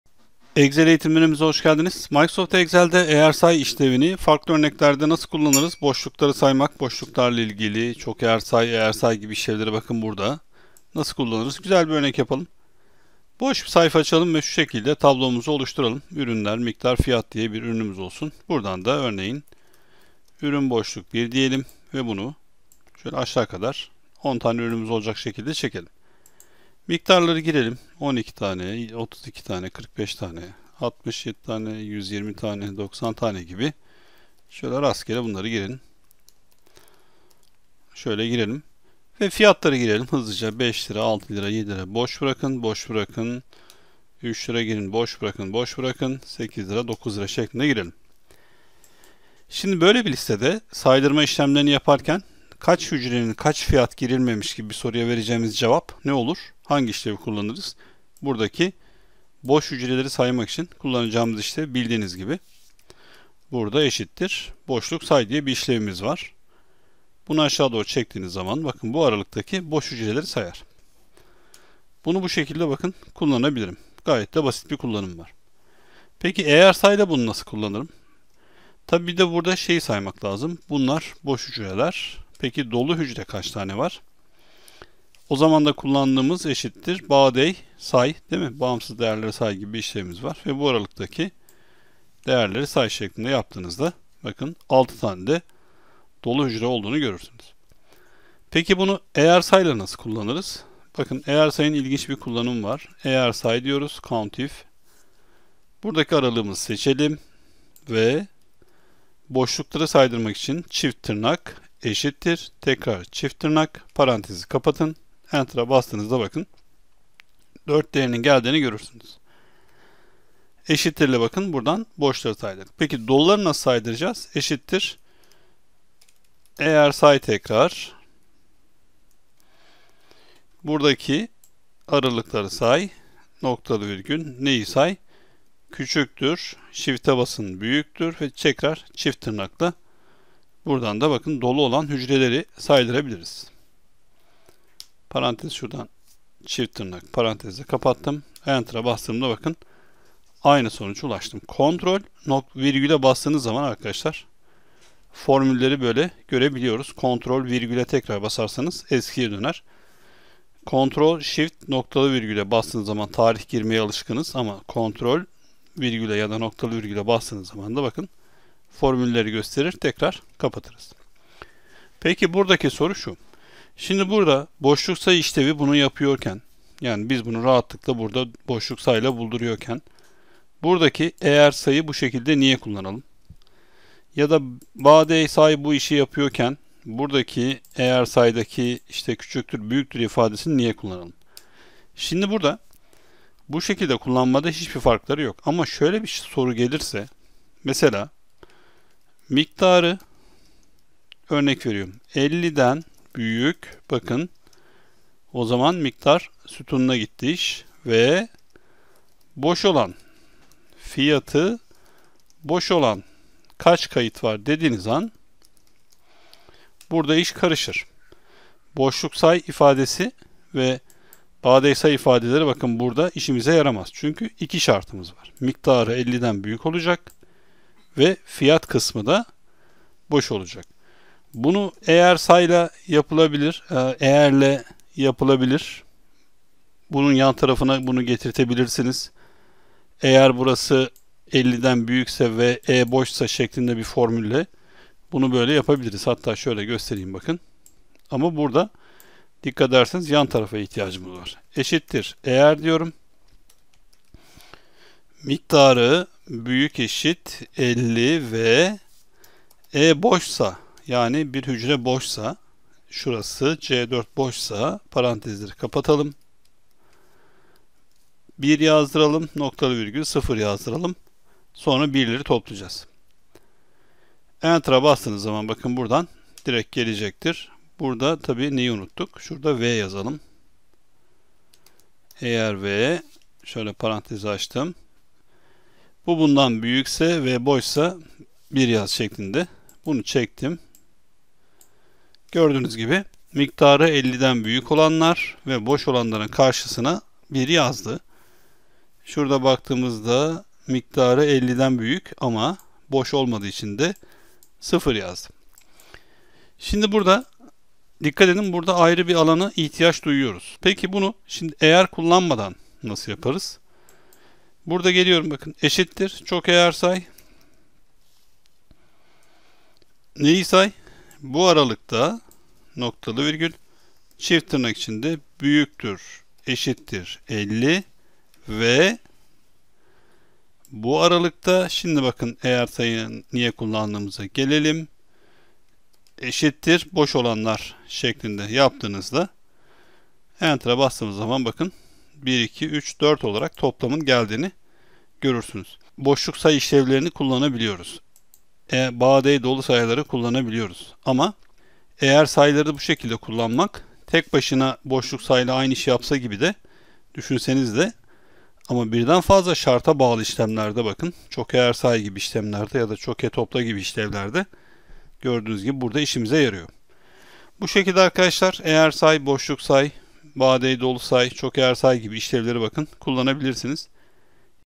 Excel Eğersay ile boş ve dolu hücreleri sayma nasıl yapılır- 680 | Ömer BAĞCI
- Eğersay İşlevi ile boş ve dolu hücreleri sayma nasıl yapılır. Eğersay kullanarak bir aralıktaki boş hücreleri nasıl sayarız. Eğersay kullanarak dolu hücreleri nasıl sayarız. Eğersay işlevinin farklı kullanımları. Excel eğitimleri için adresini ziyaret edebilirsiniz.

00:00 Başlangıç
00:20 Tabloyu oluşturmak
01:02 BOŞLUKSAY İşlevi
01:26 BAĞ_DEĞ_SAY İşlevi
01:46 EĞERSAY ile boşlukları saymak
02:48 Formülleri Göstermek
04:12 EĞER, EBOŞSA İşlevleri
05:15 ÇOKEĞERSAY İşlevi

İleri Düzey Kurumsal Excel Eğitimleri için adresini ziyaret ediniz.

Farklı Excel eğitimlerimiz için, vidoport üzerindeki Excel eğitimlerimize göz atın.